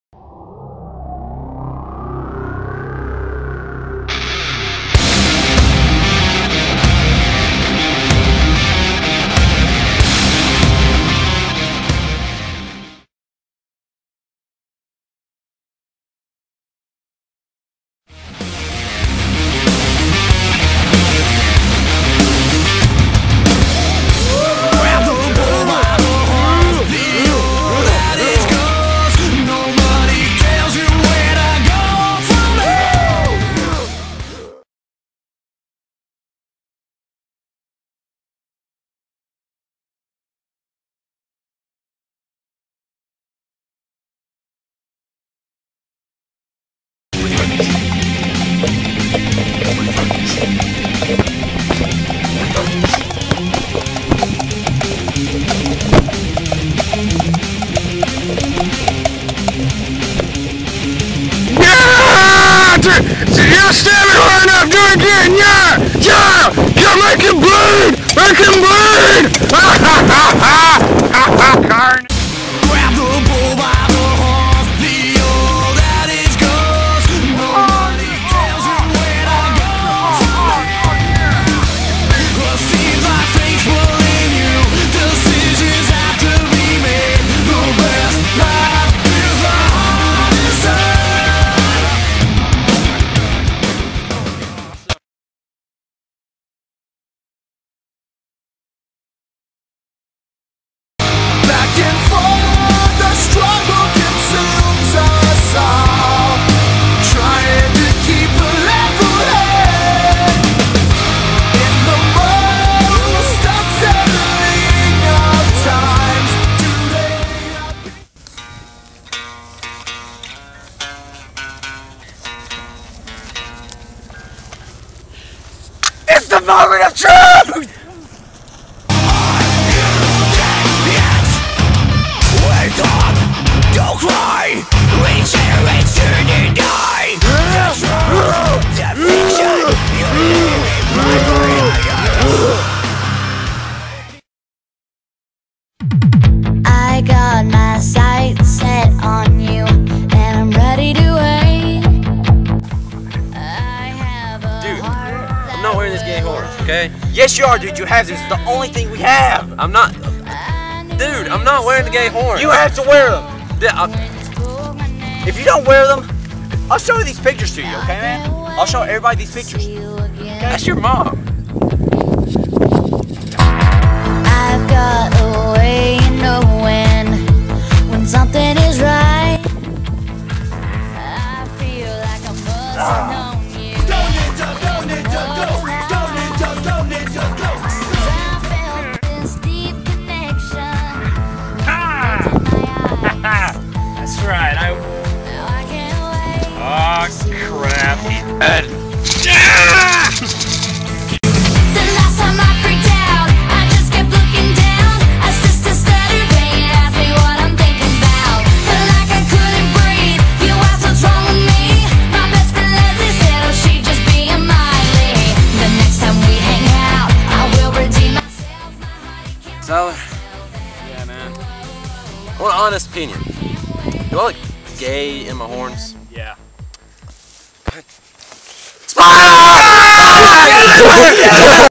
Death Metal Bull Fight
We had to do a bull fight for spanish class, this is what happened.